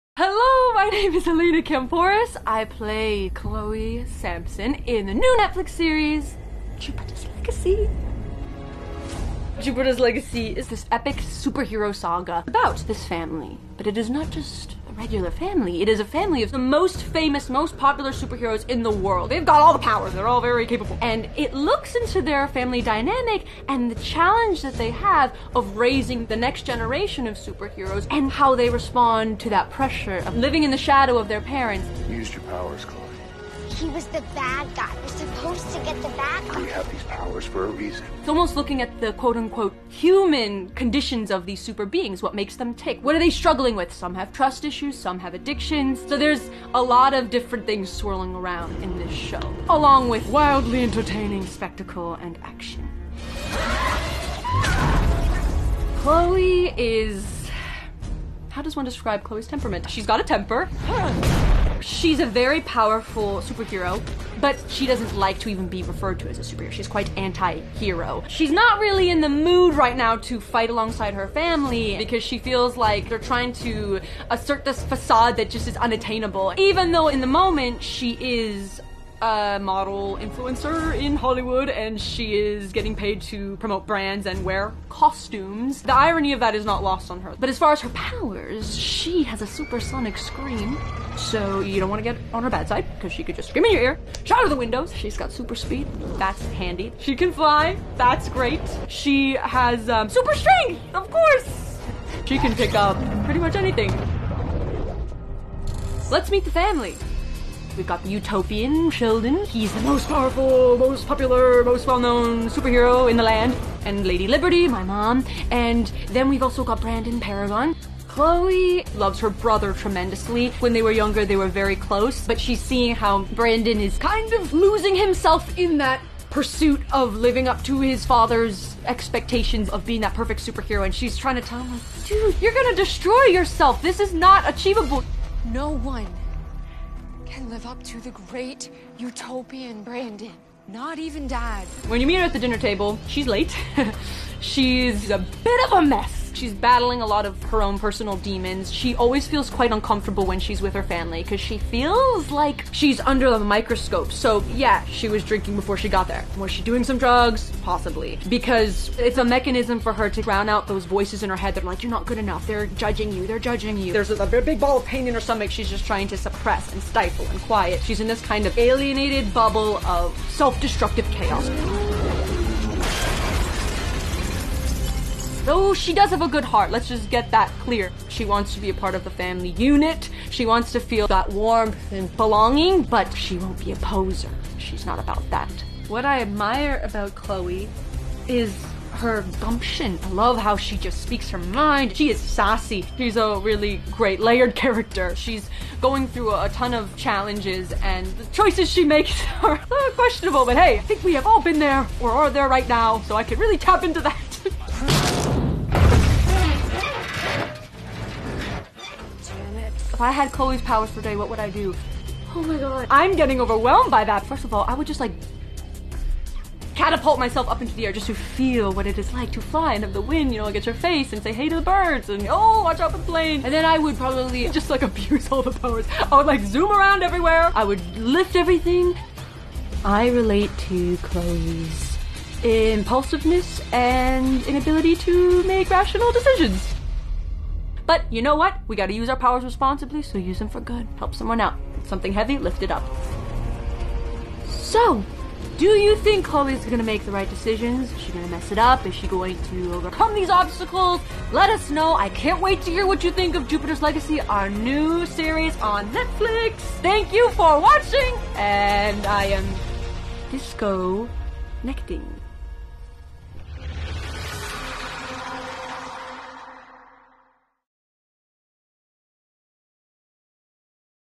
Jupiter’s Legacy’s Elena Kampouris And Her Character Chloe Sampson
Jupiter’s Legacy’s Elena Kampouris goes into all the power behind her character, Chloe